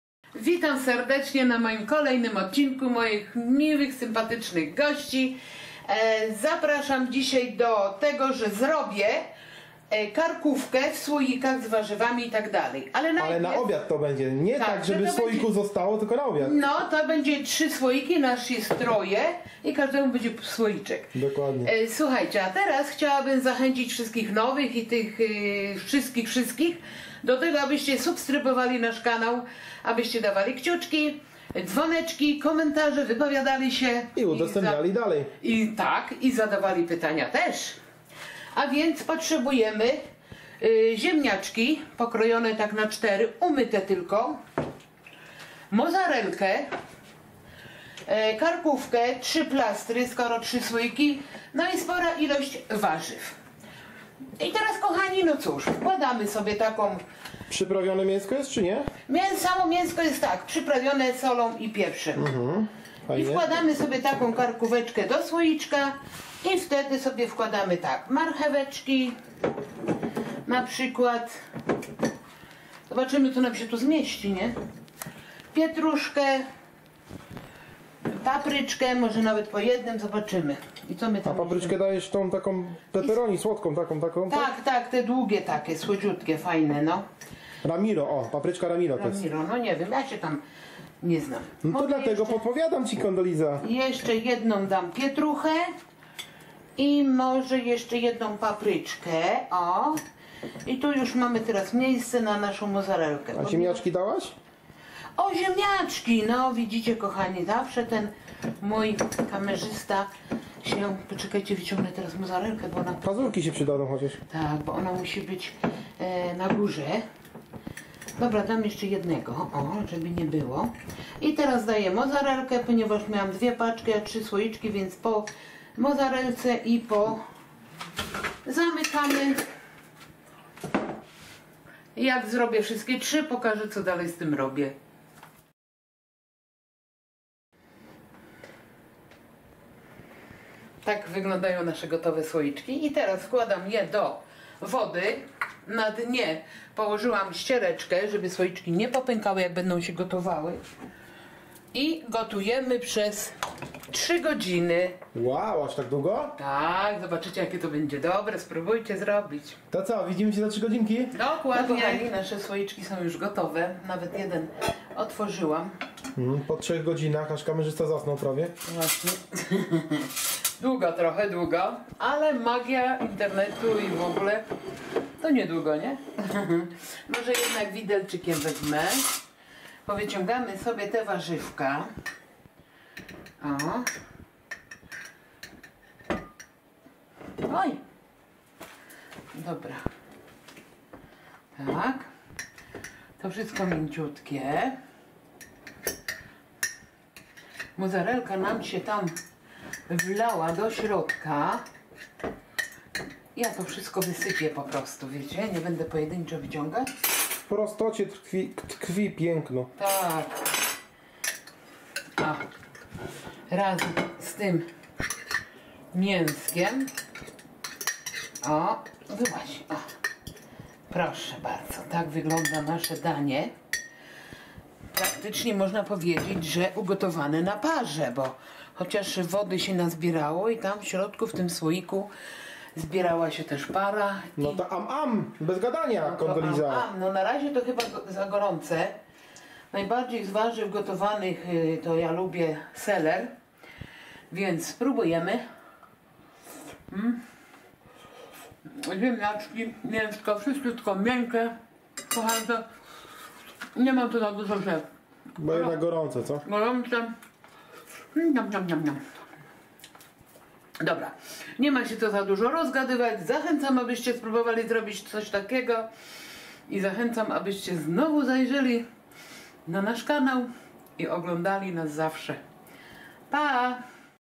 Takiej karkówki ze słoika jeszcze nie jadłeś !!! Rodzina Januszy !!!
Nie uwierzycie chociaż nie wygląda ładnie ale smakuje obłędnie !!!

SKŁADNIKI NA JEDEN SŁOIK !!
1 grubszy plaster karkówki
1 marchewek
1 pietruszka
1 papryka ramiom
mały kawałek seler
1 op mozzarelli
1 ziemniak nie obrany ale myty : -)
he he no i 1 słoik !!!
SMACZNEGO !!!